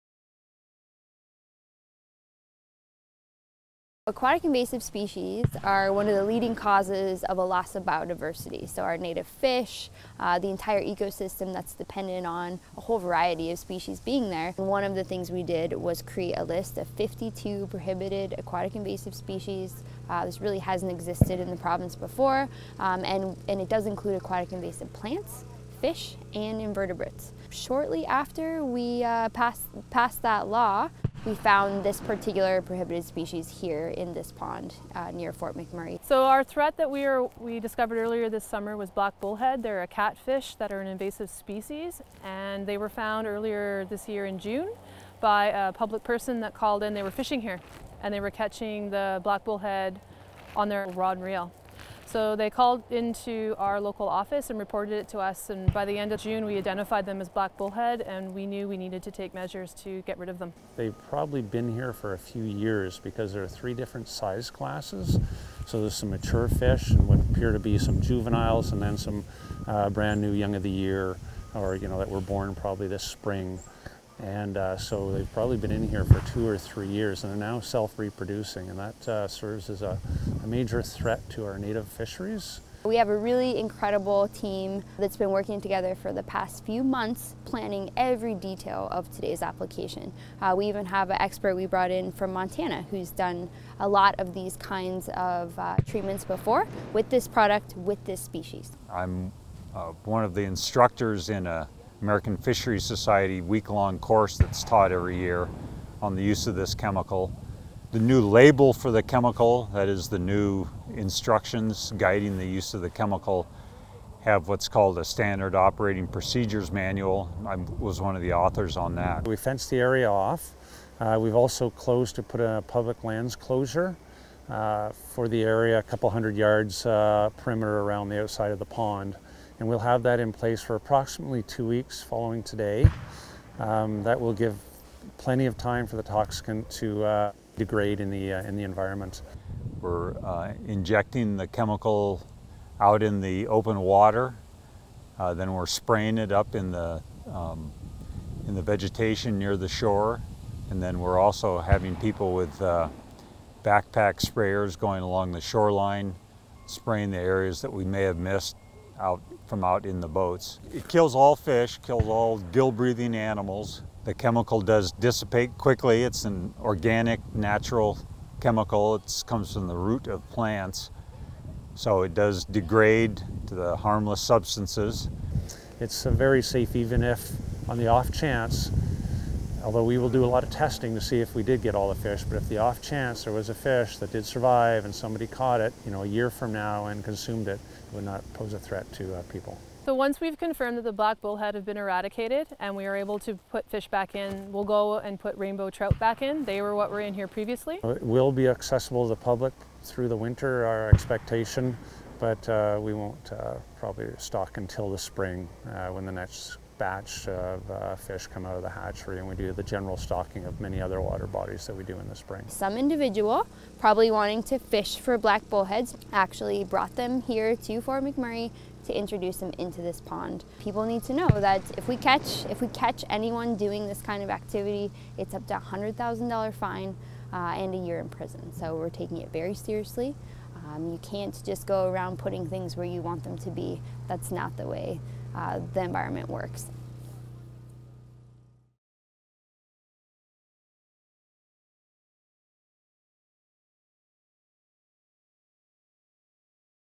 Black Bullhead Eradication
Environment and Parks staff in Fort McMurray have been battling black bullhead, an invasive fish species that was illegally introduced into Texaco East Trout Pond.